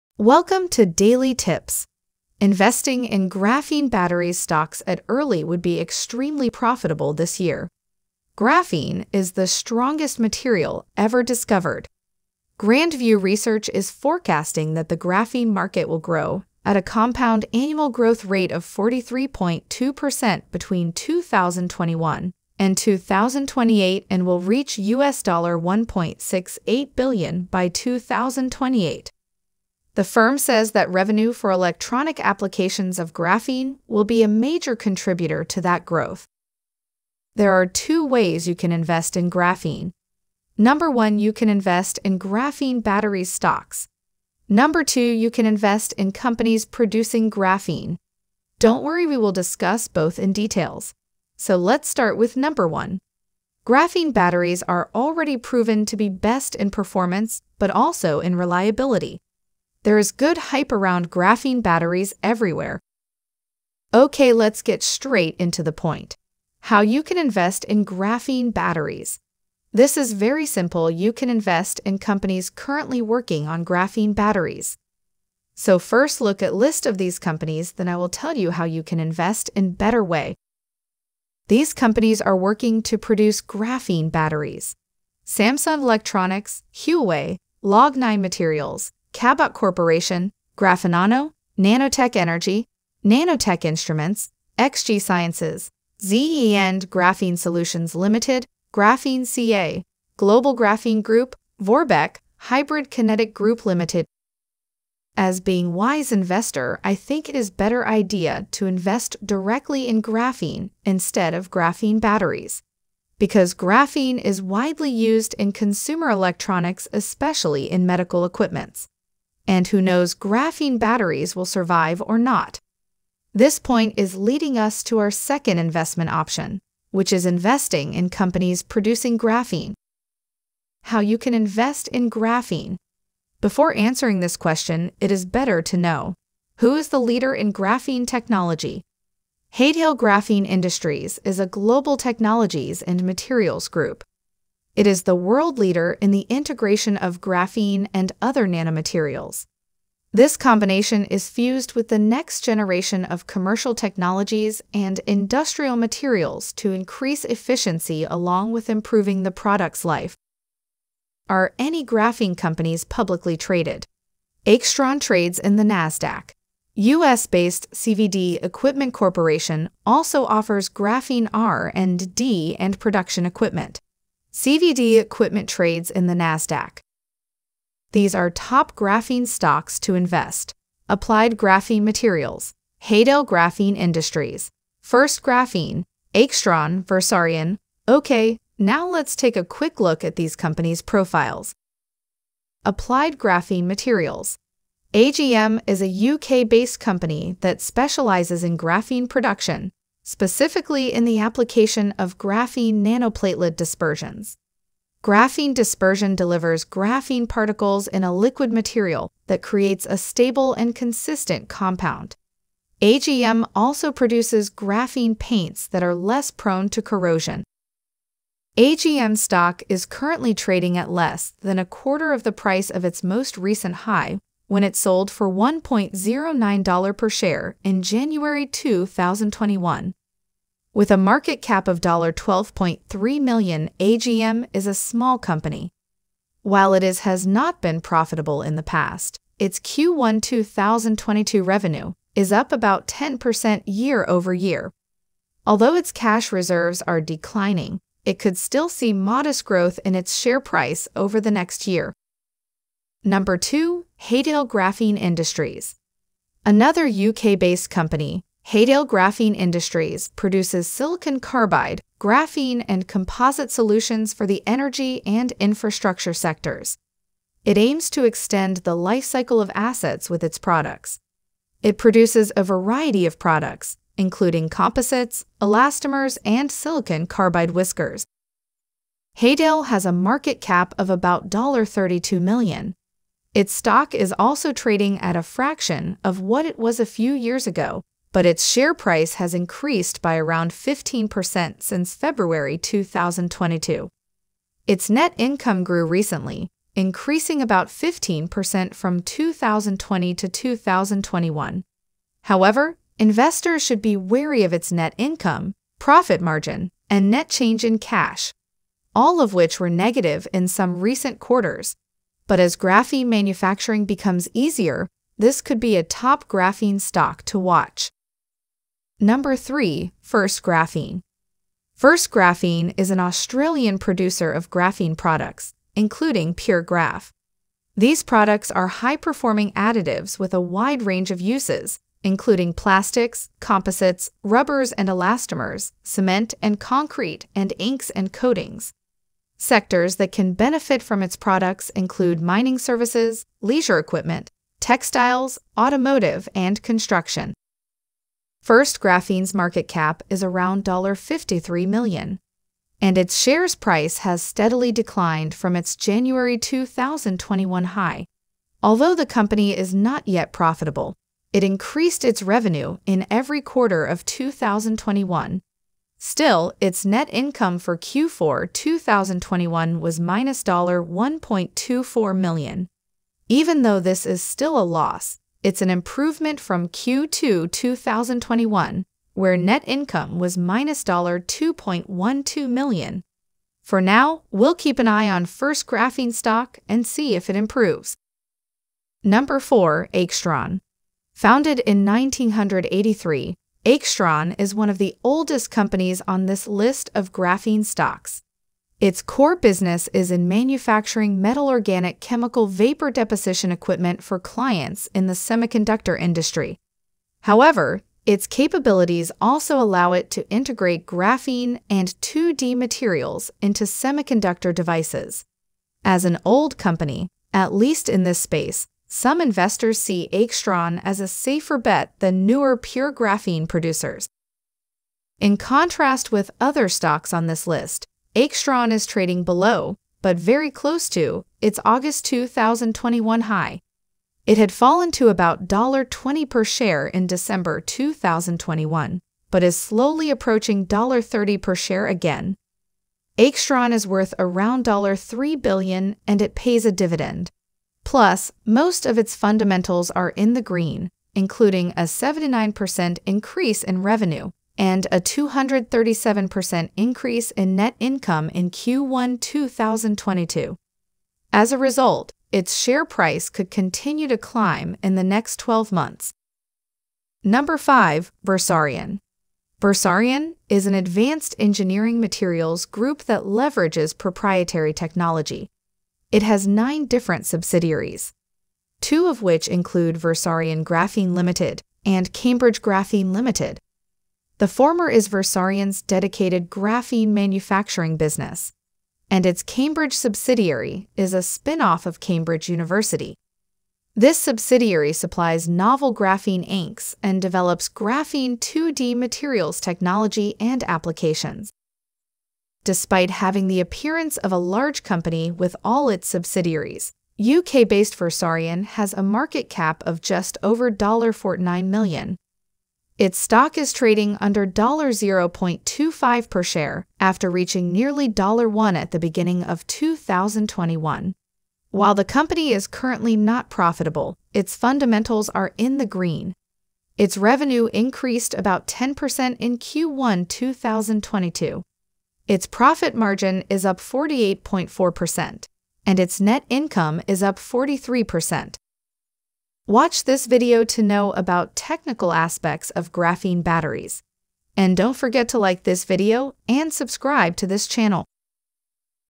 How To Invest in Graphene Battery Stocks | Best Graphene Batteries Stocks to Invest in NASDAQ
Best opportunity to invest in graphene and in graphene battery stocks. In this video, you will know how to invest in graphene batteries and which stocks are best to invest in graphene. I have briefly described some good graphene stocks in Nasdaq.

Also, I have shared some financial reports and talked about some growing graphene battery companies. After the Graphene battery market analysis I have also listed the five best graphene manufacturing companies for investment opportunities.